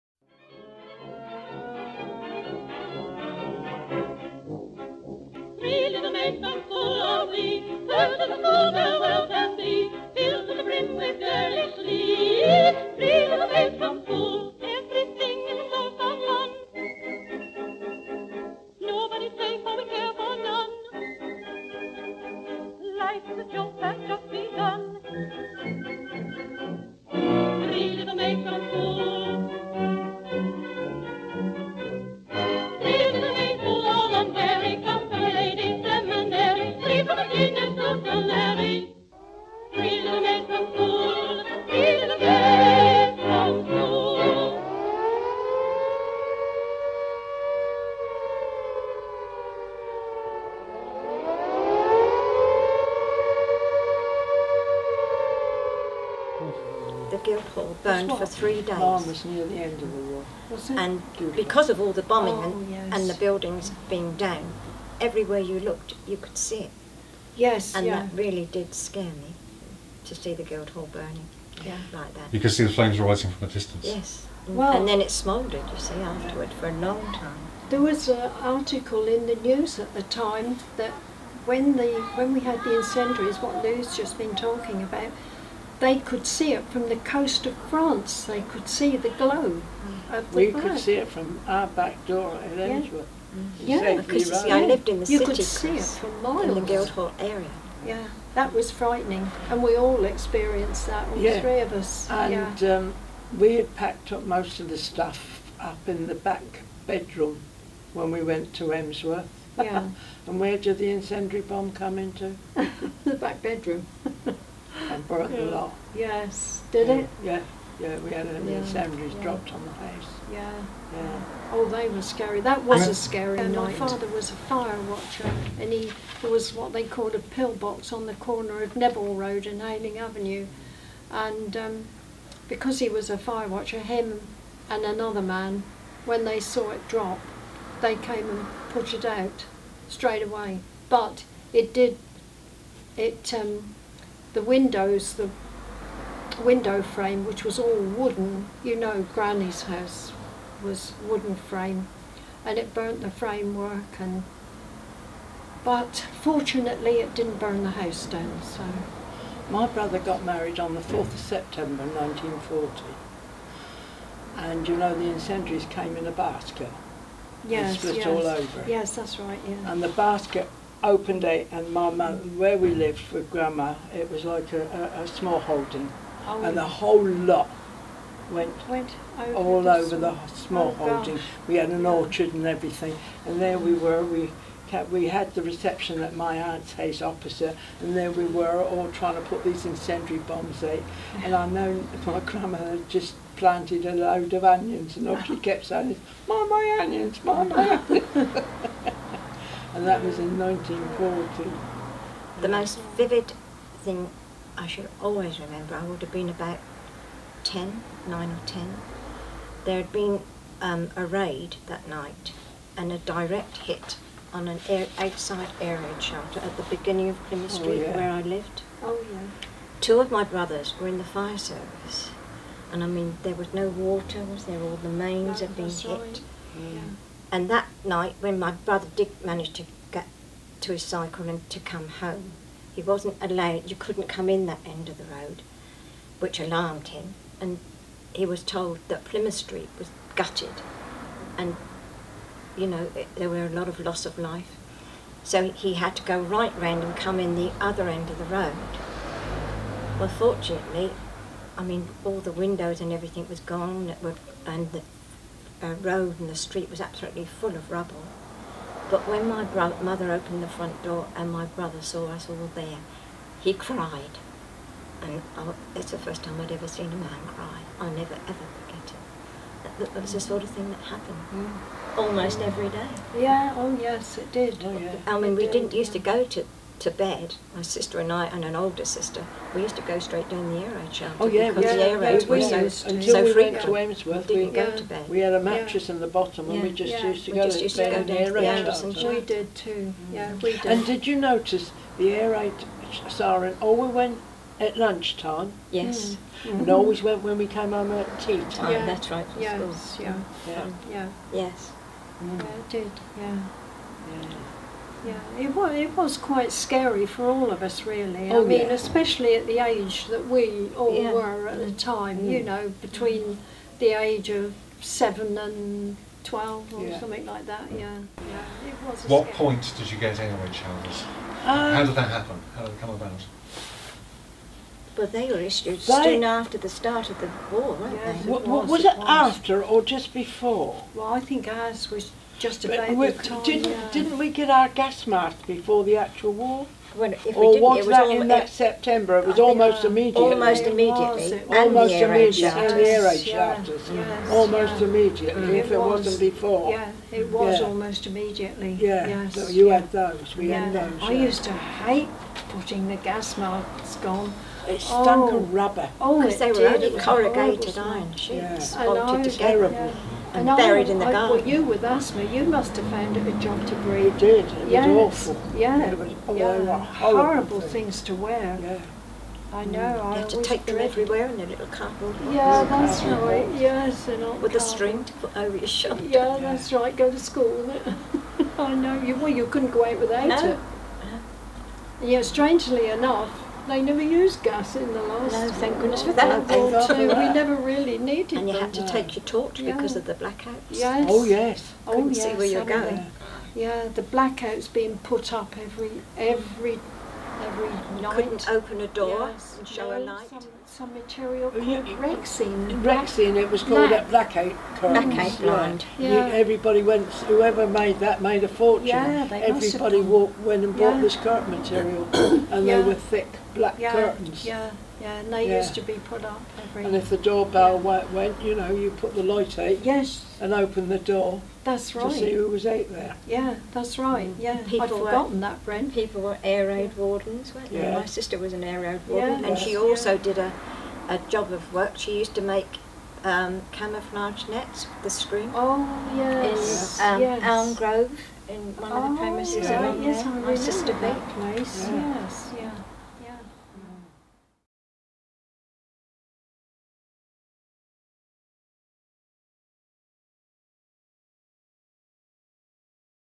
A Wartime Childhood in Portsmouth Part 6 : the Blitz
Part 6 of a 8 part sequence of short video conversations about growing up in wartime Portsmouth.